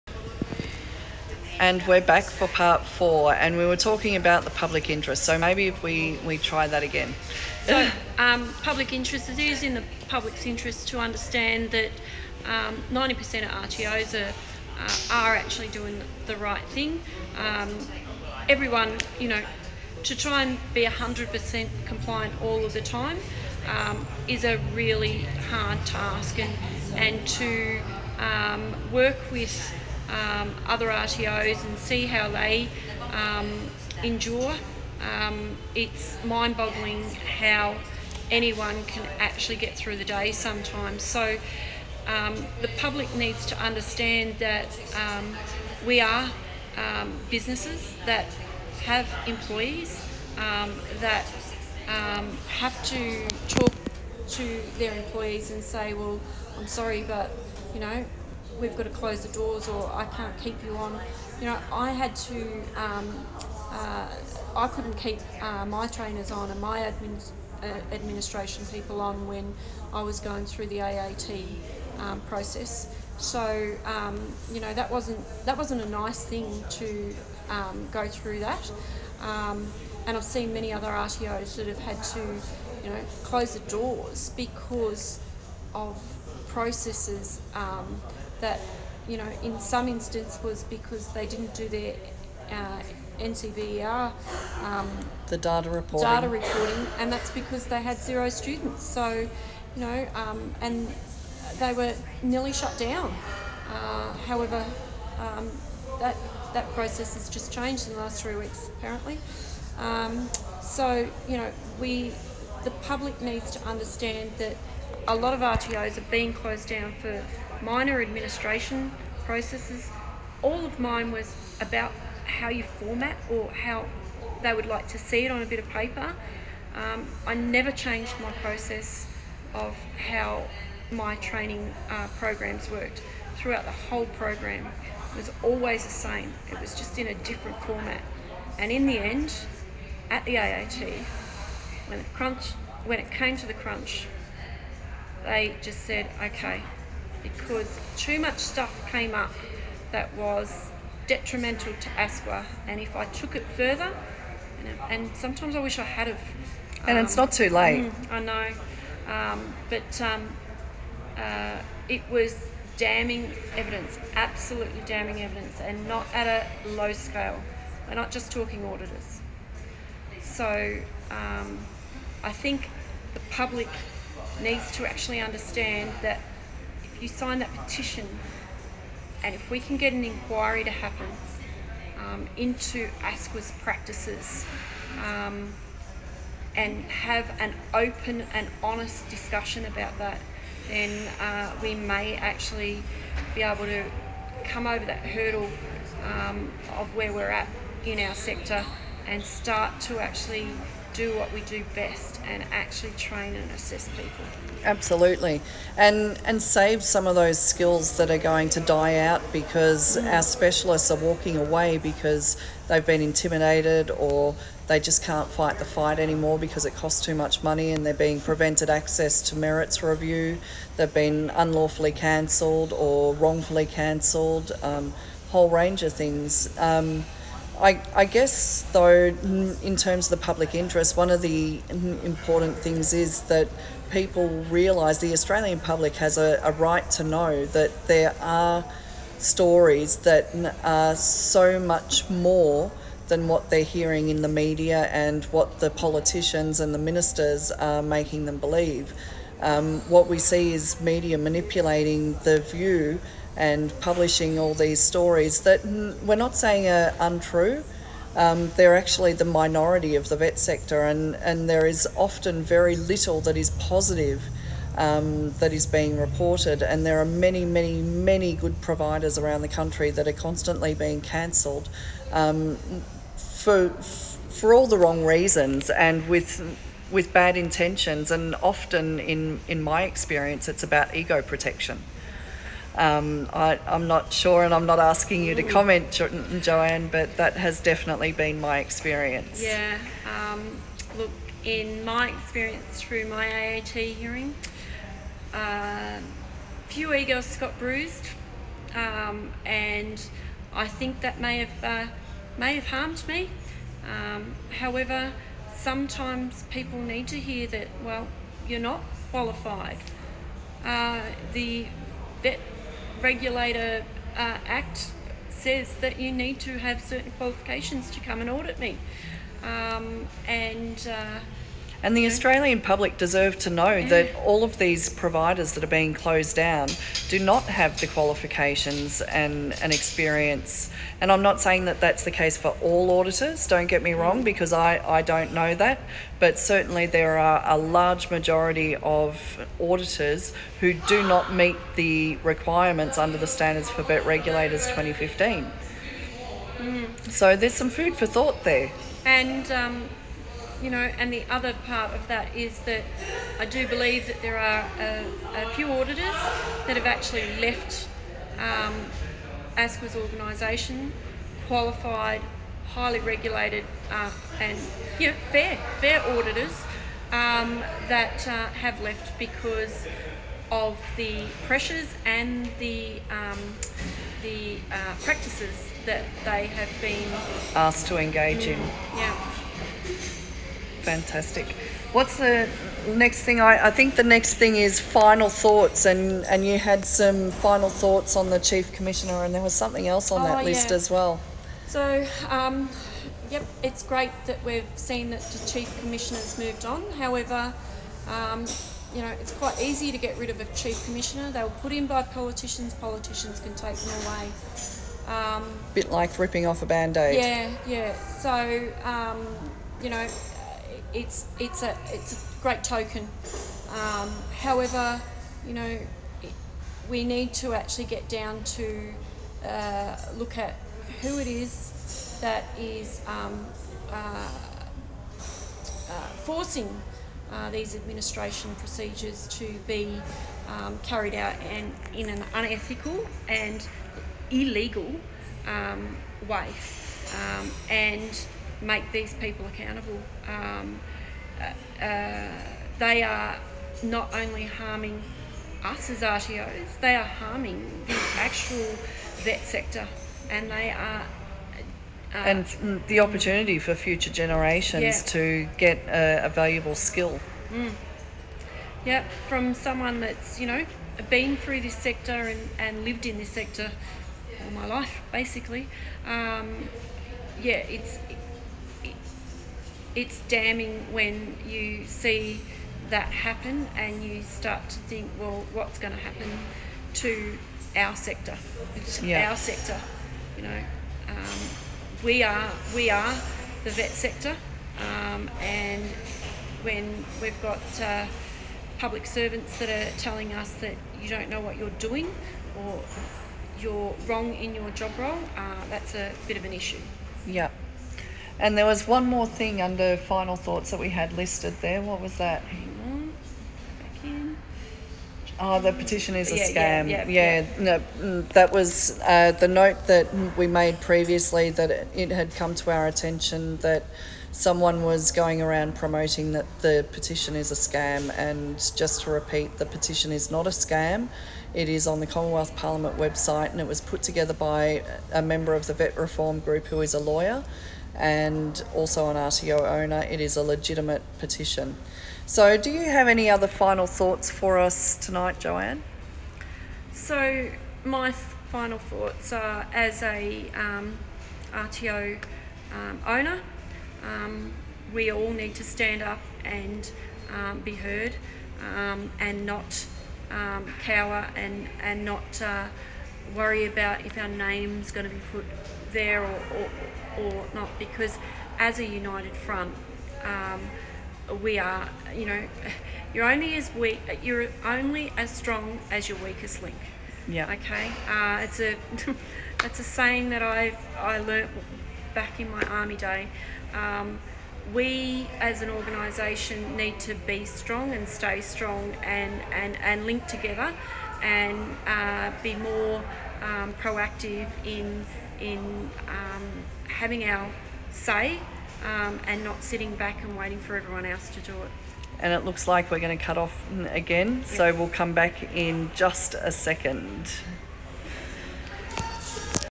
ASQA Exposed! Australia's VET sector: The Other Side of the Story - Joanne Cornelius - Part 4
This 5 part live video with Joanne Cornelius is 1 of the reasons why Australia's National VET Regulator, the Australian Skills Quality Authority MUST be investigated.

Joanne takes us through some of her journey in resurrecting her RTO after wrongdoing by ASQA.  She discusses some of the massive concerns that the public never hears about such as the conflicts of interest with ASQA staff, the breaches of:
- The Australian Public Service Code of Conduct;
- The Standards for VET Regulators 2015;
Legal Services Directions 2015 (Model Litigant Obligation) and more!

Joanne's courage in speaking out and exposing the truth in her long and costly journey to fairness is both mindblowing and heartbreaking.